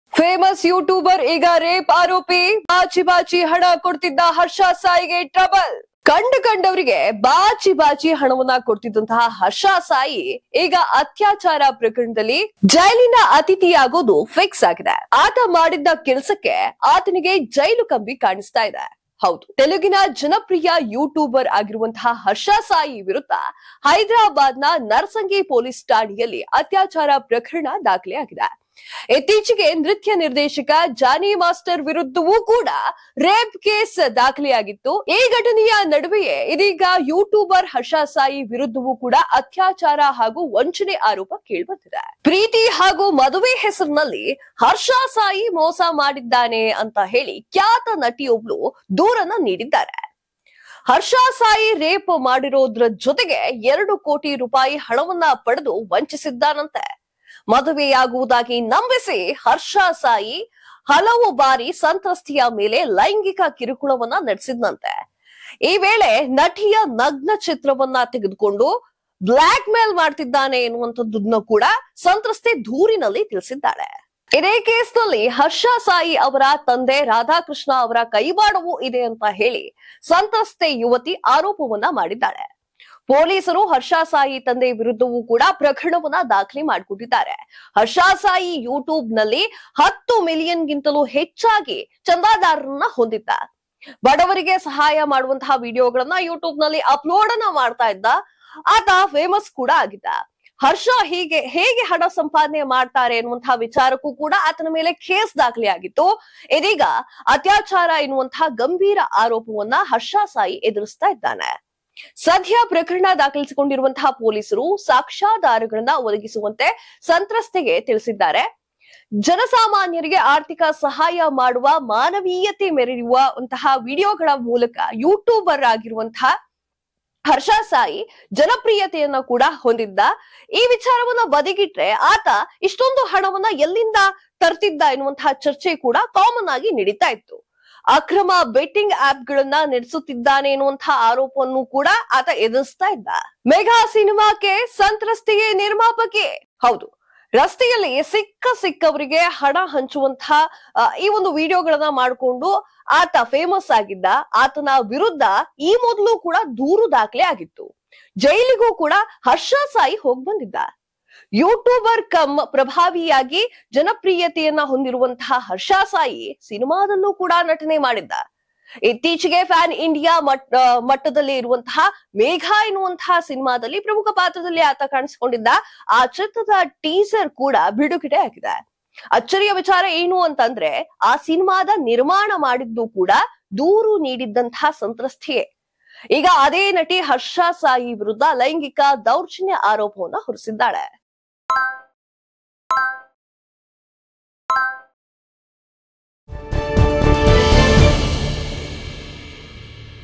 ಯೂಟ್ಯೂಬರ್‌ ಹರ್ಷ ಸಾಯಿ ವಿರುದ್ಧ ರೇ*ಪ್‌ ಕೇಸ್‌! | ಆ ಪ್ರಖ್ಯಾತ ನಟಿಗೆ ಬ್ಲ್ಯಾಕ್​ಮೇಲ್! | Harsha Sai | KTVNews
ಯೂಟ್ಯೂಬರ್‌ ಹರ್ಷ ಸಾಯಿ ವಿರುದ್ಧ ರೇ*ಪ್‌ ಕೇಸ್‌! | ಆ ಪ್ರಖ್ಯಾತ ನಟಿಗೆ ಬ್ಲ್ಯಾಕ್​ಮೇಲ್! | Harsha Sai | Content Creator | Social Worker | Karnataka TV News